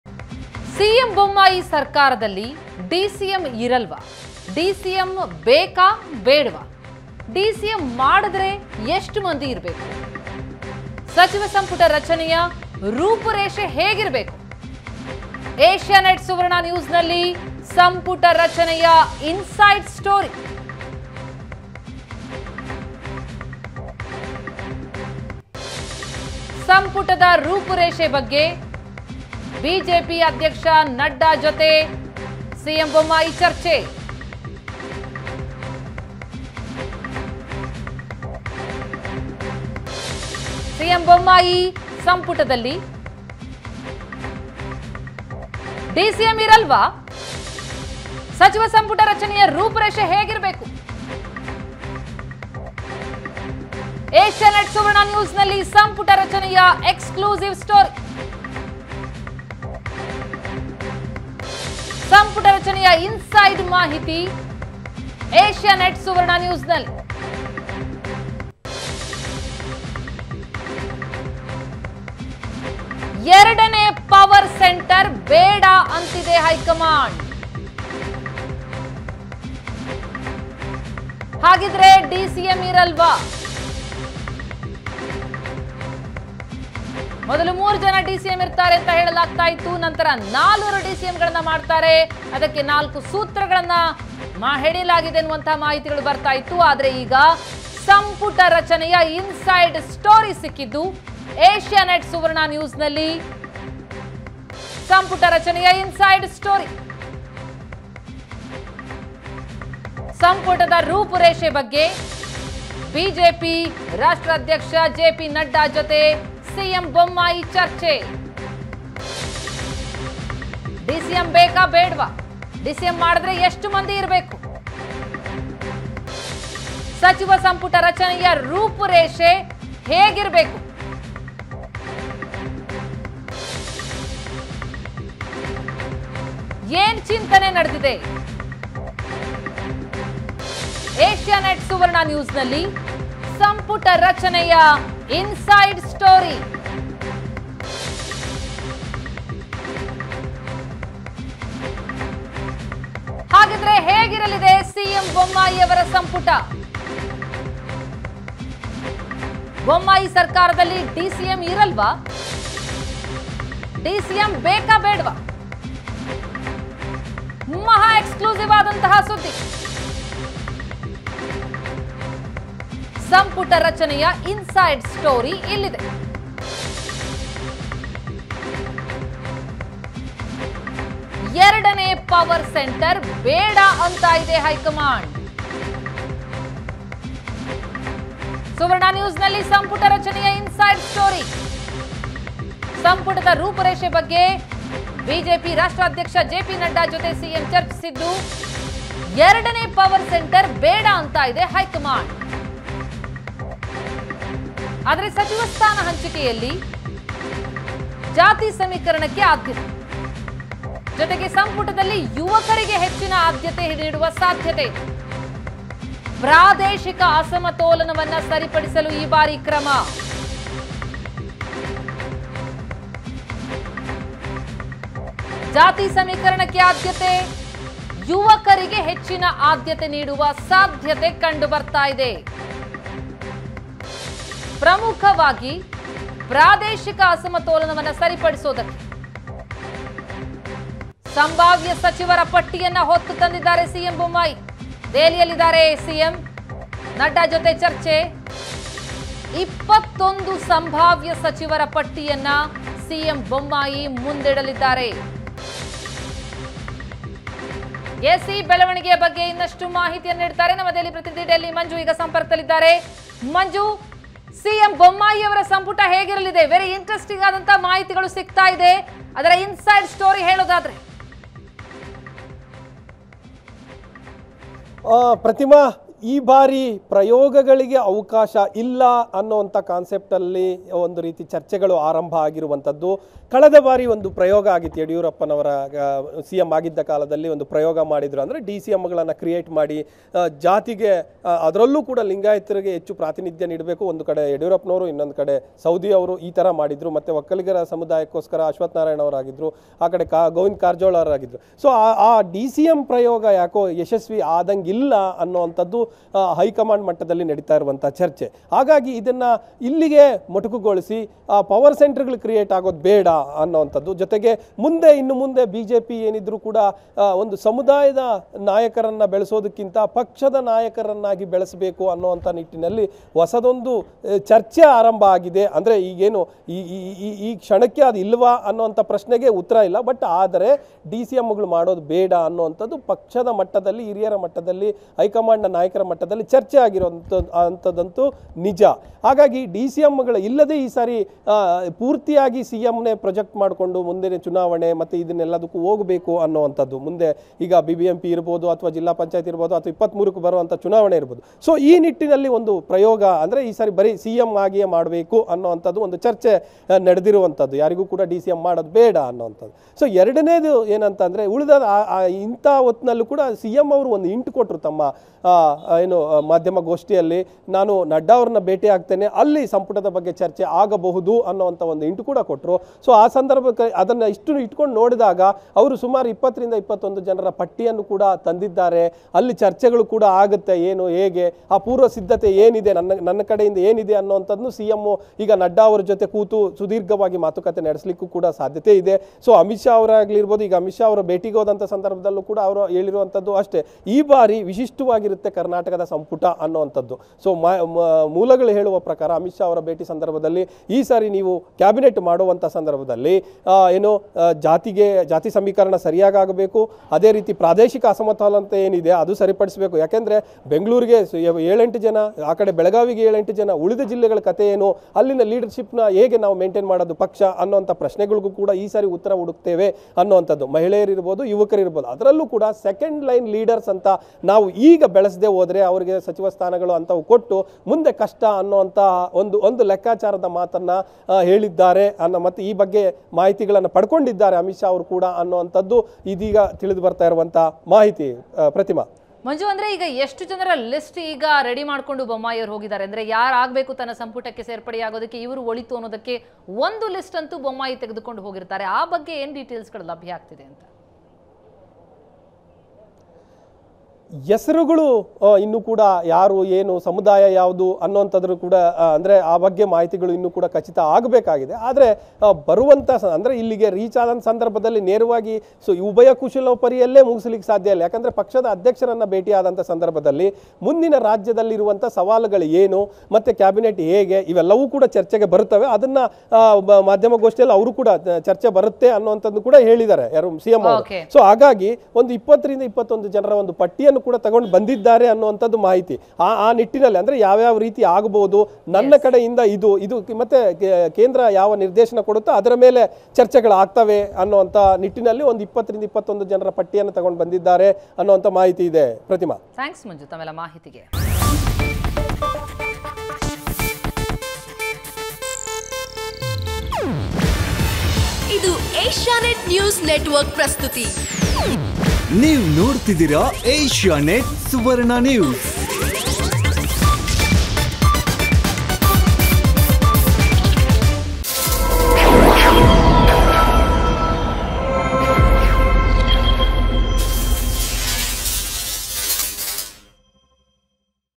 ಸಿಎಂ ಬೊಮ್ಮಾಯಿ ಸರ್ಕಾರದಲ್ಲಿ ಡಿಸಿಎಂ ಇರಲ್ವಾ ? ಸಂಪುಟ ರಚನೆಯ ರೂಪುರೇಷೆ ಹೇಗಿರುತ್ತೆ ? | Karnataka New Cabinet
Karnataka New Cabinet | Basavaraj Bommai | ಸಿಎಂ ಬೊಮ್ಮಾಯಿ ಸರ್ಕಾರದಲ್ಲಿ ಡಿಸಿಎಂ ಇರಲ್ವಾ ? ಸಂಪುಟ ರಚನೆಯ ರೂಪುರೇಷೆ ಹೇಗಿರುತ್ತೆ ? | Karnataka New Cabinet